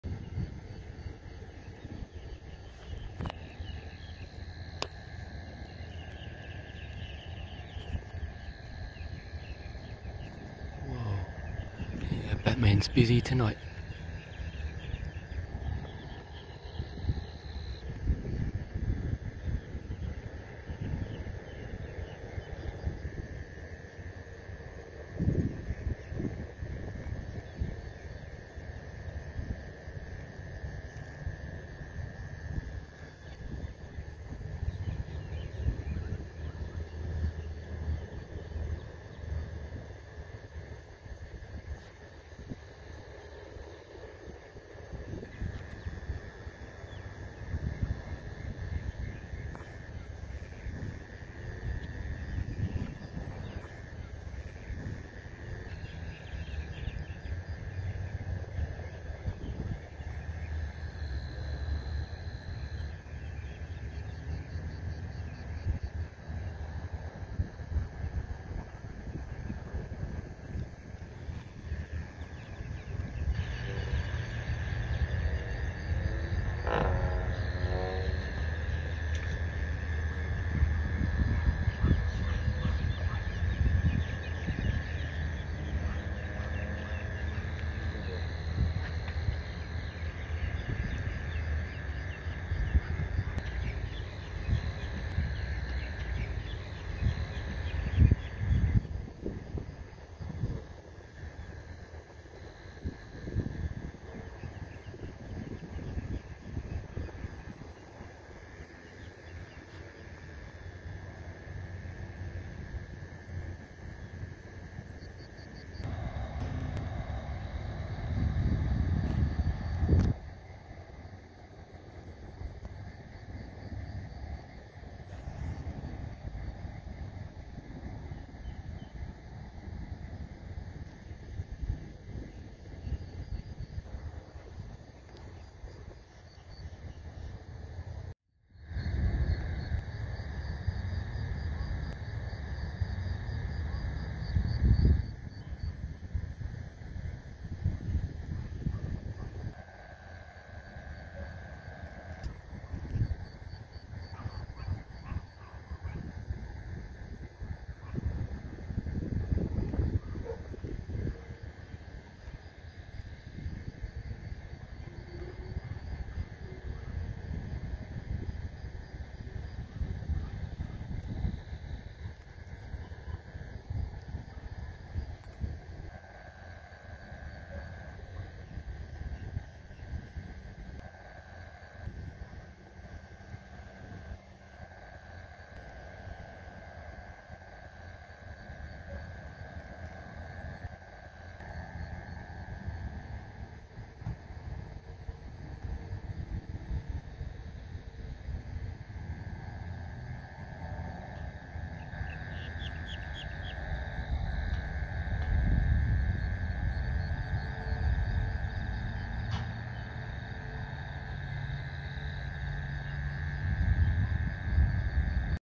HALLOWEEN And The Bats Are Everywhere
Nature timed it perfectly, the bats are out right on time...HALLOWEEN!